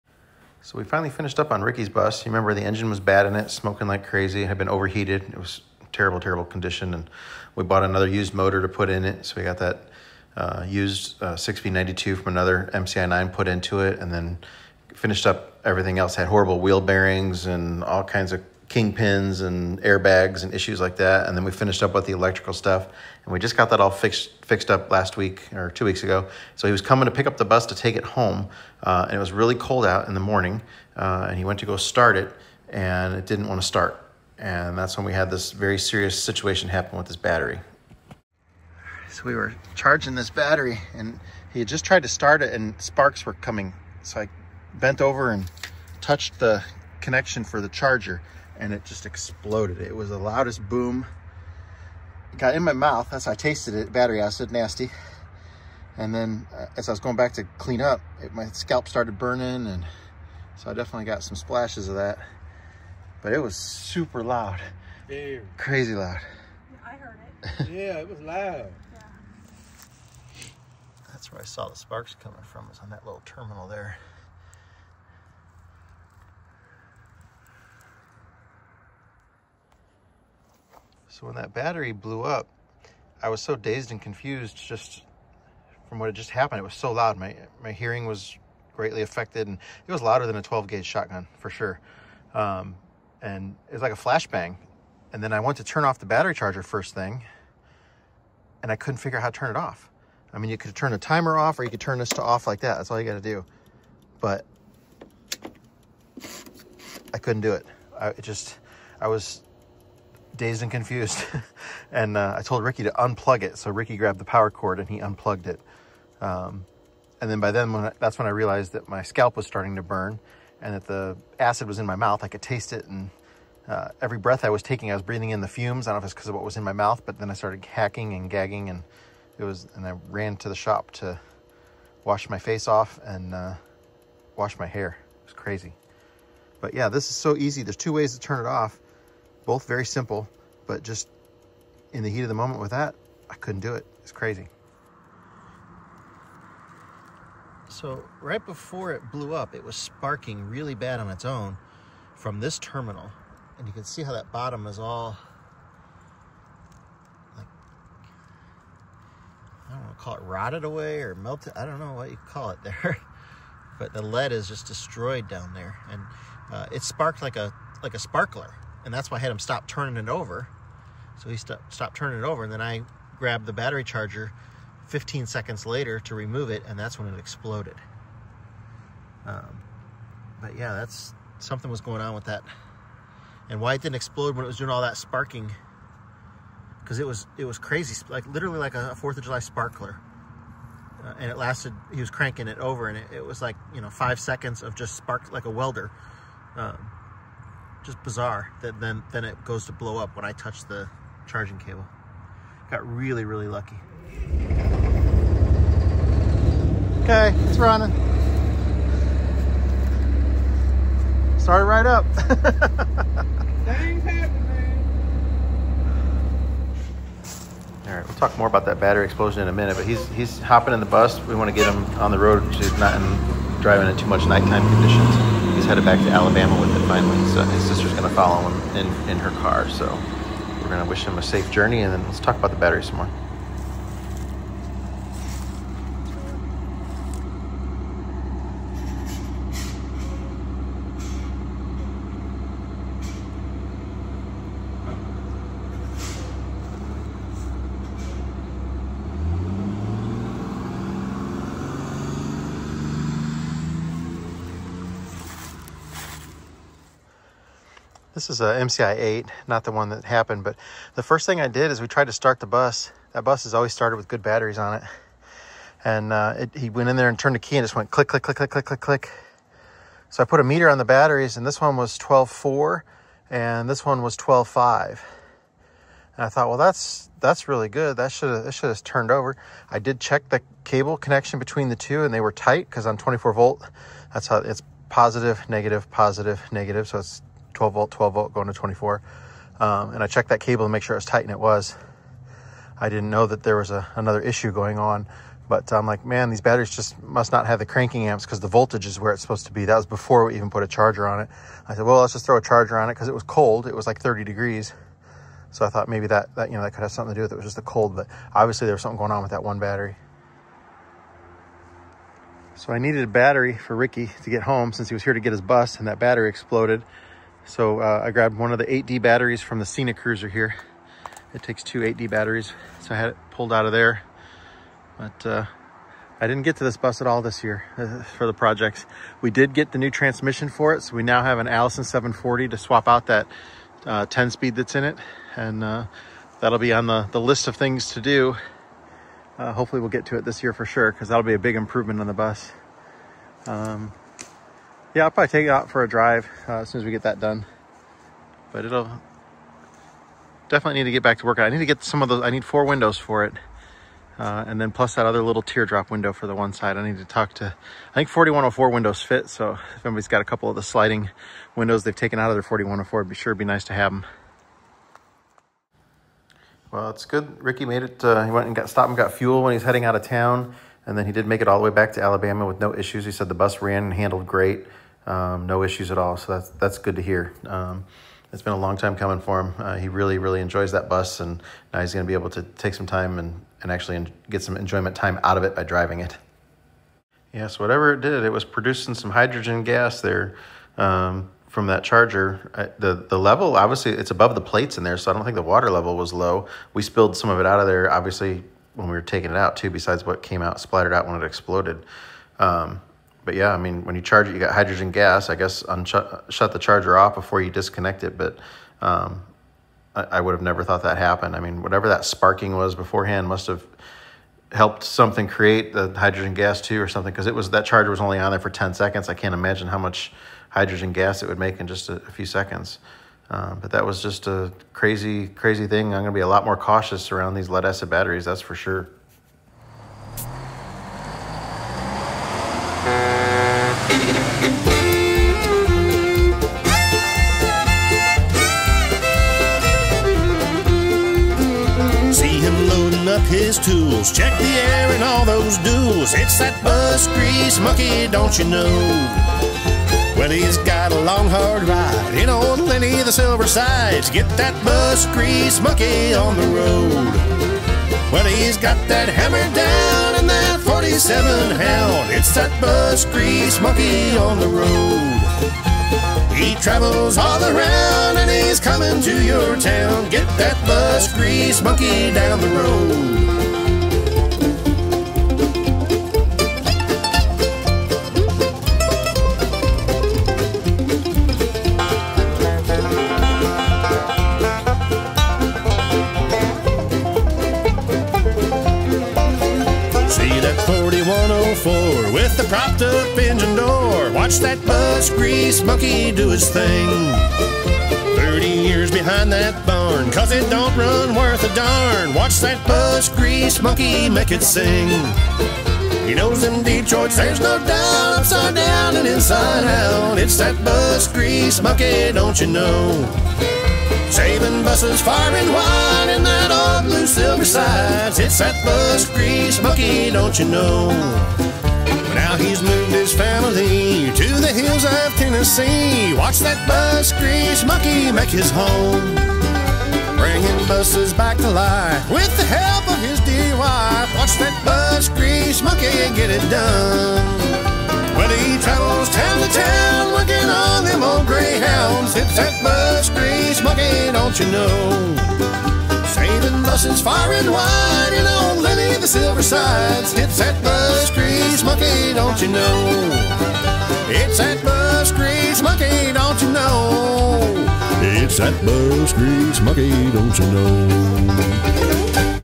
Lead acid battery exploded in my face.
This giant 8D size bus battery exploded with a very loud bang just as I removed the battery charger.  A 12v battery hooked in series to 24v in the MCi mc9 bus.  It sparked badly when the engine was turning over and about ten seconds later it exploded when i went to remove the battery charger.